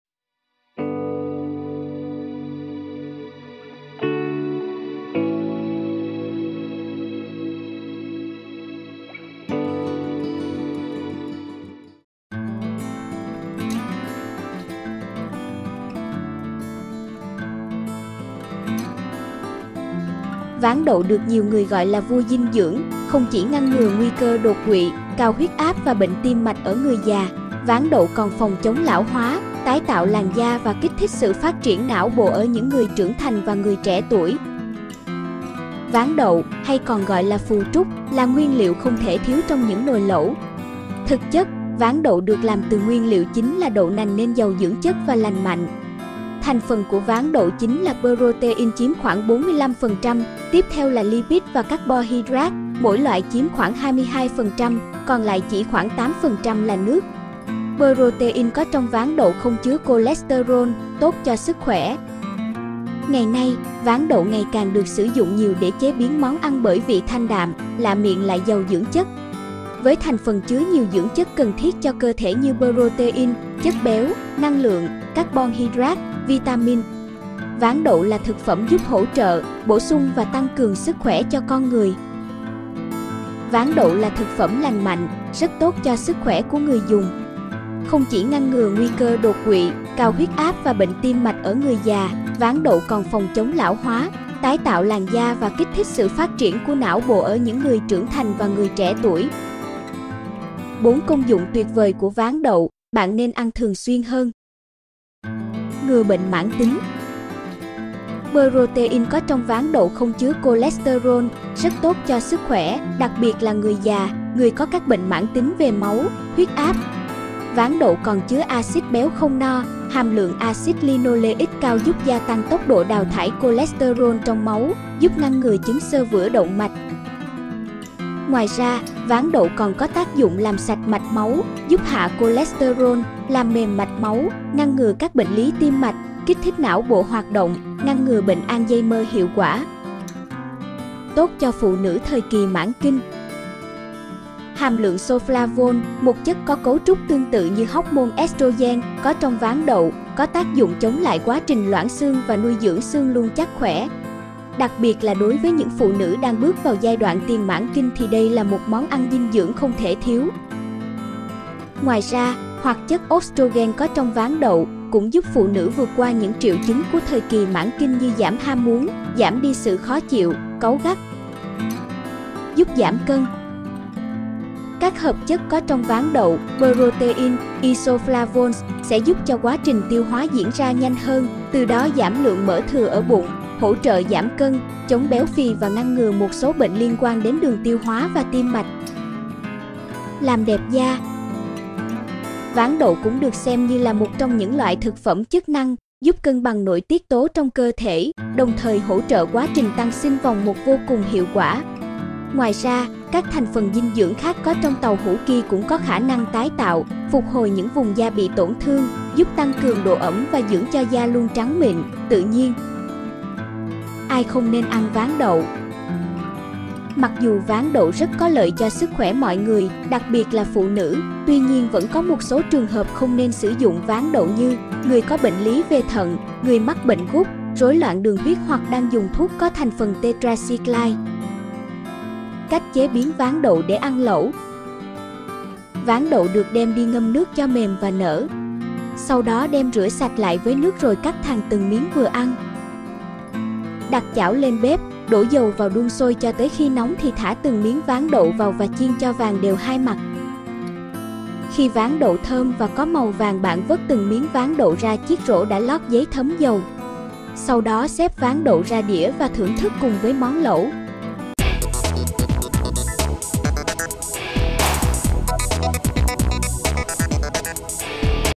Thứ thường dùng ăn lẩu, giúp làm sạch máu và ngừa bệnh tim mạch hiệu quả || Sức Khỏe - Ẩm Thực
4 công dụng tuyệt vời của váng đậu, bạn nên ăn thường xuyên hơn

Ngừa bệnh mãn tính

Protein có trong váng đậu không chứa cholesterol, rất tốt cho sức khỏe (đặc biệt là người già, người có các bệnh mãn tính về máu, huyết áp). Váng đậu còn chứa axit béo không no, hàm lượng axit linoleic cao giúp gia tăng tốc độ đào thải cholesterol trong máu, giúp ngăn ngừa chứng xơ vữa động mạch.

Ngoài ra, váng đậu còn có tác dụng làm sạch mạch máu, giúp hạ cholesterol, làm mềm mạch máu, ngăn ngừa các bệnh lý tim mạch, kích thích não bộ hoạt động, ngăn ngừa bệnh Alzheimer hiệu quả,…

Tốt cho phụ nữ thời kì mãn kinh

Hàm lượng soflavone (một chất có cấu trúc tương tự như Hoocmon estrogen) có trong váng đậu có tác dụng chống lại quá trình loãng xương và nuôi dưỡng xương luôn chắc khỏe. Đặc biệt là đối với những phụ nữ đang bước vào giai đoạn tiền mãn kinh thì đây là một món ăn dinh dưỡng không thể thiếu.

Ngoài ra, hoạt chất ostrogen có trong váng đậu cũng giúp phụ nữ vượt qua những triệu chứng của thời kì mãn kinh như giảm ham muốn, giảm đi sự khó chịu, cáu gắt,…

Giúp giảm cân

Các hợp chất có trong váng đậu (Protein, isoflavones,…) sẽ giúp cho quá trình tiêu hóa diễn ra nhanh hơn, từ đó giảm lượng mỡ thừa ở bụng, hỗ trợ giảm cân, chống béo phì và ngăn ngừa một số bệnh liên quan đến đường tiêu hóa và tim mạch…

Làm đẹp da

Váng đậu cũng được xem như là một trong những loại thực phẩm chức năng giúp cân bằng nội tiết tố trong cơ thể, đồng thời hỗ trợ quá trình tăng sinh vòng 1 vô cùng hiệu quả. Ngoài ra, các thành phần dinh dưỡng khác có trong tàu hũ ky cũng có khả năng tái tạo, phục hồi những vùng da bị tổn thương, giúp tăng cường độ ẩm và dưỡng cho da luôn trắng mịn, tự nhiên.